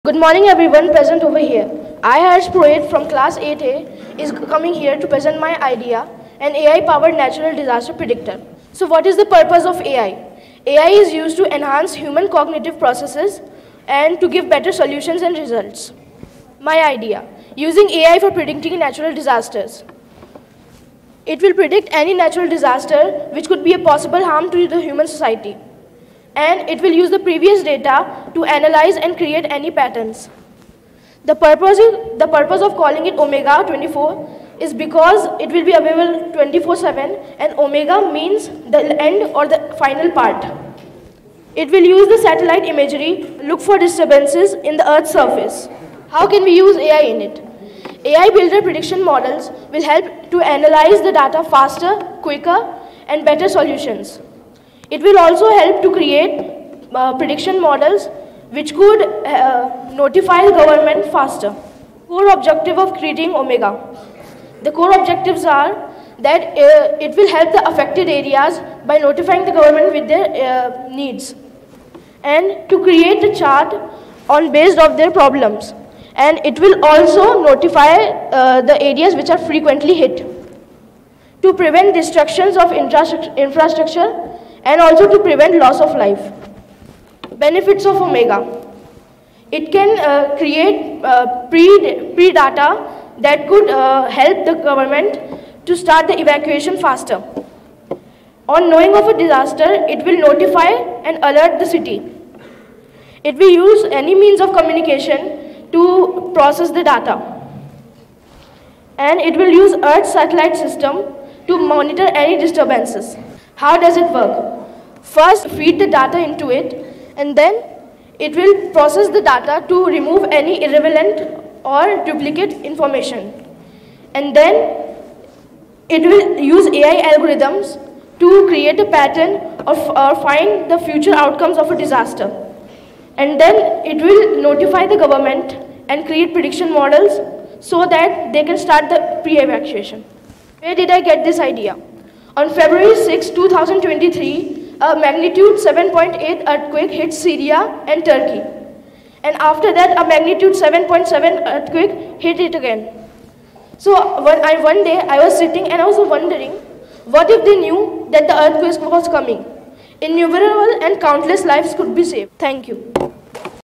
Harsh Purohit Presents Omega: AI-Powered Natural Disaster Predictor | Data and AI Talks Raipur 2023
Step into the future of disaster management with Harsh Purohit from Krishna Public School Nava Raipur, as he takes the stage at Data and AI Talks Raipur 2023 to introduce his groundbreaking AI-powered creation - "Omega: AI-Powered Natural Disaster Predictor."

In this awe-inspiring session, Harsh Purohit reveals his visionary innovation, Omega, an extraordinary AI system designed to revolutionize the way we anticipate and respond to natural disasters. Drawing from his profound passion for technology and a deep understanding of data analysis, Harsh has crafted Omega to be more than a tool; it's a lifeline for communities at risk.

Omega transcends the conventional by harnessing the power of AI to predict natural disasters with unprecedented accuracy. Harsh delves into the technical intricacies behind Omega, exploring its advanced machine learning algorithms, data integration capabilities, and neural network architecture that empower it to process vast amounts of data from multiple sources and generate actionable insights.

Witness Harsh Purohit's journey of innovation, from conceptualizing Omega to harnessing the potential of AI for the greater good. His commitment to leveraging technology for disaster mitigation shines through in every aspect of Omega's design.

Whether you're a tech enthusiast, a data aficionado, or simply concerned about the impact of natural disasters, Harsh Purohit's presentation is a must-watch. Join us at Data and AI Talks Raipur 2023 to witness the unveiling of Omega and gain a deeper understanding of the brilliant mind behind this game-changing AI-powered natural disaster predictor.

We eagerly anticipate your presence at this momentous event!